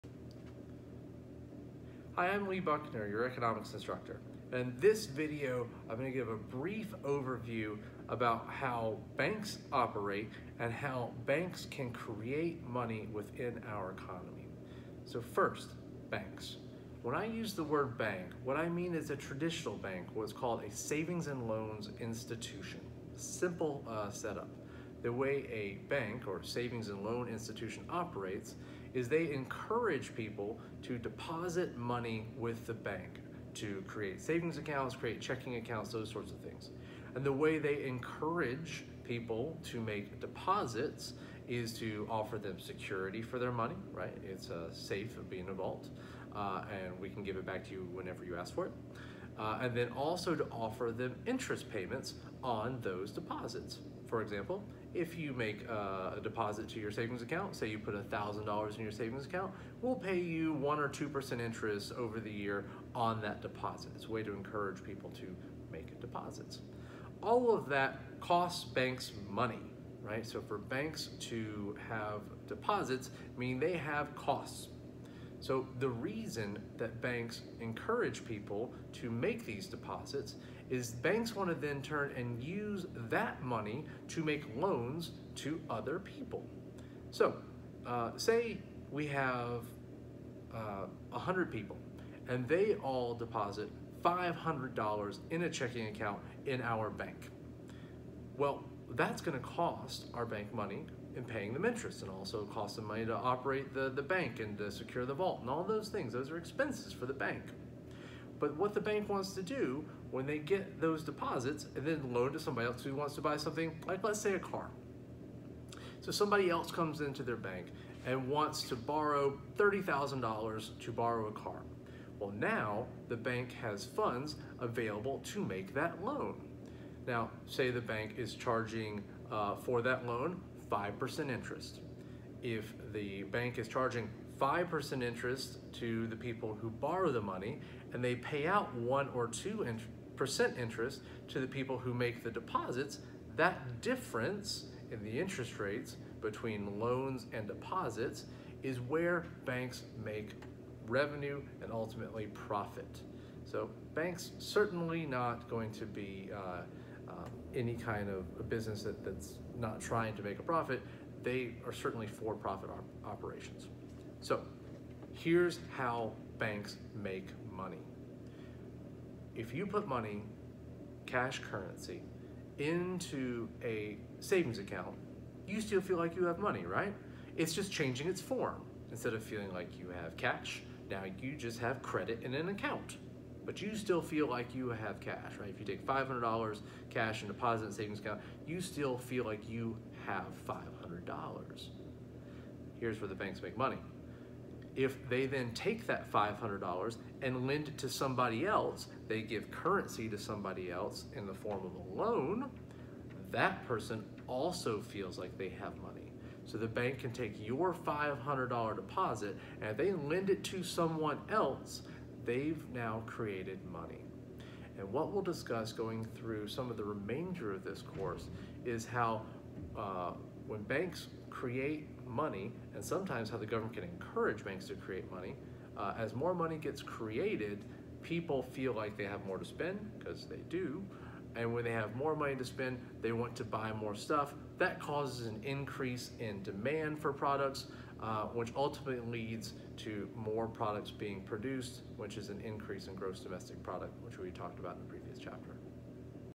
Buckner Banks and Money Creation Video
Video discussing how savings and loans institutions (basic/traditional banks) operate and how they can create new money by lending out deposits.